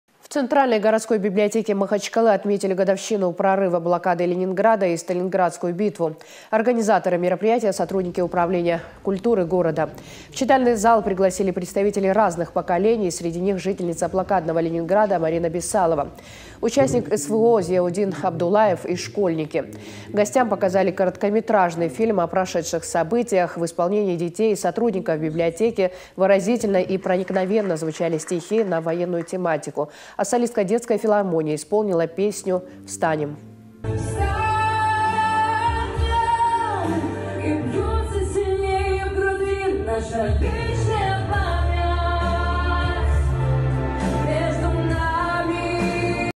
В Махачкале отметили годовщину прорыва блокады Ленинграда и Сталинградскую битву
В центральной городской библиотеке Махачкалы отметили годовщину прорыва блокады Ленинграда и Сталинградскую битву. Организаторы мероприятия сотрудники Управления культуры города.  В читальный зал пригласили представителей разных поколений, среди них – жительница блокадного Ленинграда Марина Бессалова, участник СВО Зиявудин Абдулаев и школьники.